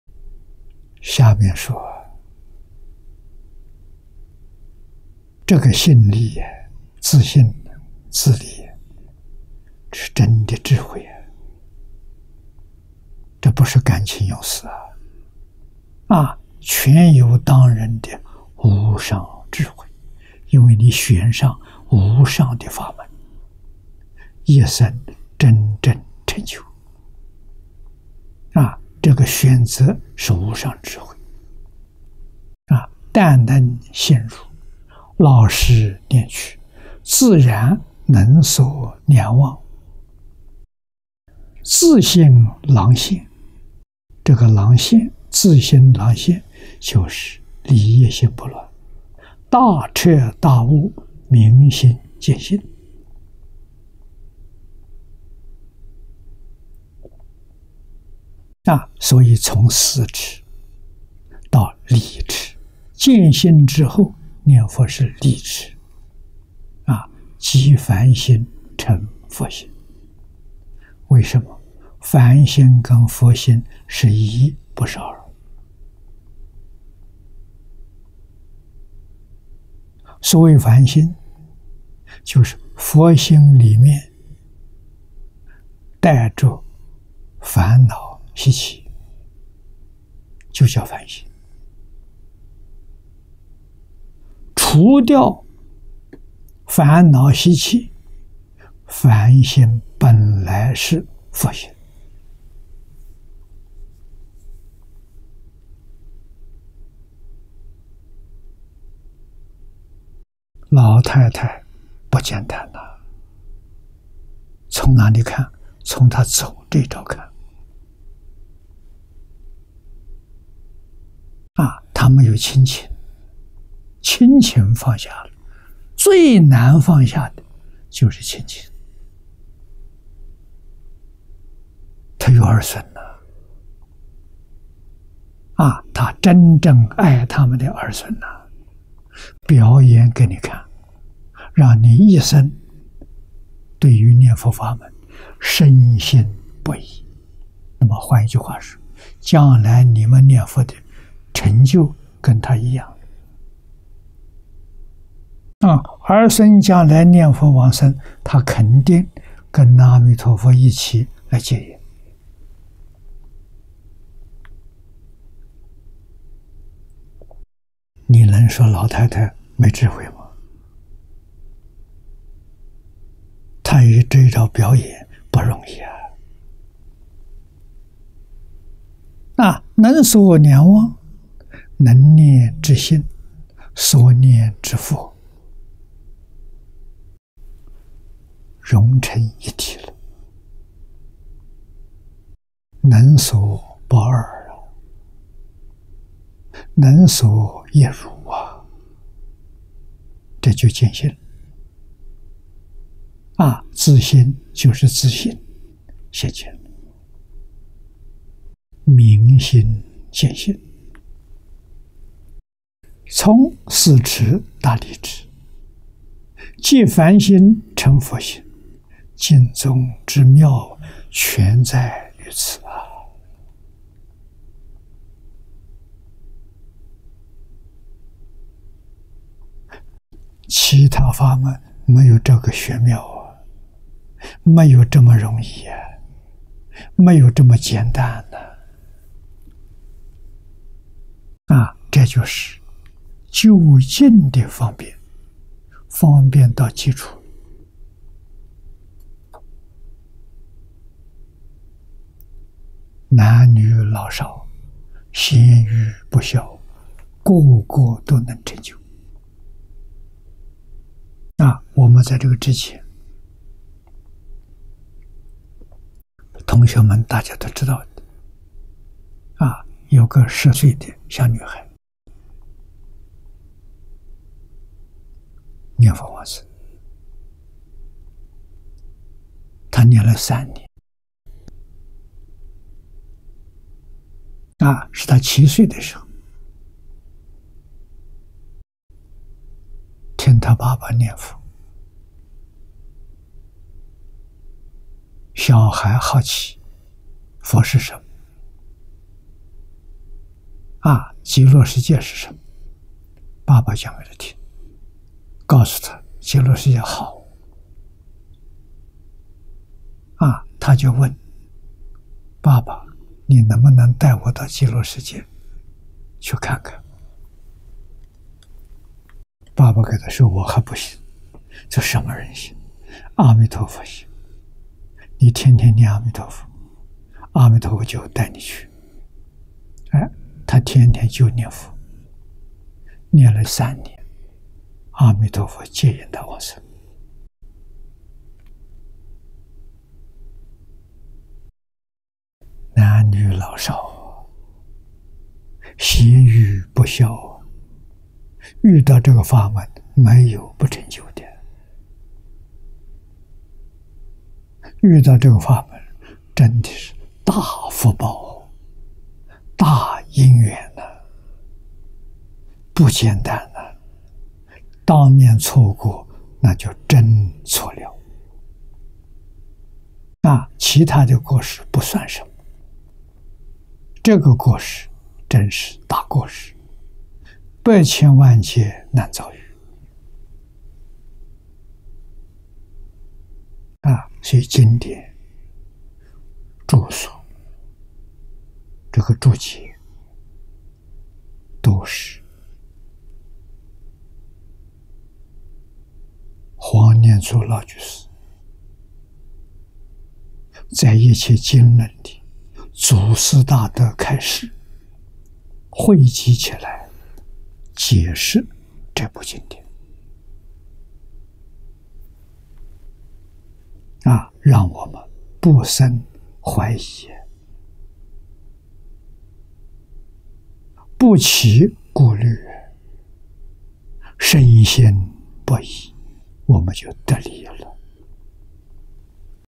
淨空老法師｜大福報 大因緣千萬別錯過
下面說，這個信力，自信、自力，是真的智慧，這不是感情用事。全由當人的無上智慧，因為你選上無上的法門，一生真正成就，這個選擇是無上智慧。
「但能信入，老實念去，自然能所兩忘，自心朗現」，這個自心朗現，就是理一心不亂，大徹大悟，明心見性。

所以，「從事持達理持」，見性之後，念佛是理持，「即凡心成佛心」，為什麼？凡心跟佛心是一不是二。
所謂凡心，就是佛心裡面帶著煩惱習氣，就叫凡心；除掉煩惱習氣，凡心本來是佛心。

老太太不簡單，從哪裡看？從她走這一招看，她沒有親情，親情放下了，最難放下的就是親情。她有兒孫，她真正愛她的兒孫，表演給你看，讓你一生對於念佛法門深信不疑。
換句話說，將來你們念佛的成就跟她一樣，兒孫將來念佛往生，她肯定跟阿彌陀佛一起來接引。你能說老太太沒智慧嗎？她以這一招表演，不容易。

「能所兩忘」，能念之心，所念之佛，融成一體，能所不二，能所一如，這就見性，自心就是自性，現前了，明心見性。「從事持達理持，即凡心成佛心。淨宗之妙，全在於此」。
其他法門沒有這個玄妙，沒有這麼容易，沒有這麼簡單，這就是究竟的方便，方便到極處。男女老少，賢愚不肖，各個都能成就。

我們在這個之前，同學們大家都知道的，有個十歲的小女孩，念佛往生，她念了三年。是她七歲的時候聽她爸爸念佛，小孩好奇，佛是什麼？極樂世界是什麼？
爸爸講給她聽，告訴她極樂世界好。她就問：「爸爸，你能不能帶我到極樂世界去看看？」爸爸給她說，我還不行。

她說什麼人行？「阿彌陀佛行，妳天天念阿彌陀佛，阿彌陀佛就會帶妳去」。她天天就念佛，念了三年，阿彌陀佛接引她往生。
男女老少，賢愚不肖，遇到這個法門沒有不成就的。遇到這個法門，真的是大福報、大因緣，不簡單，當面錯過那就真錯了。

其他的過失不算什麼，這個過失真是大過失，百千萬劫難遭遇。
所以經典、註疏，這個註解都是黃念祖老居士在一切經論裡，祖師大德開示會集起來，解釋這部經典，讓我們不生懷疑，不起顧慮，深信不疑，我們就得利益了。

2015/1/5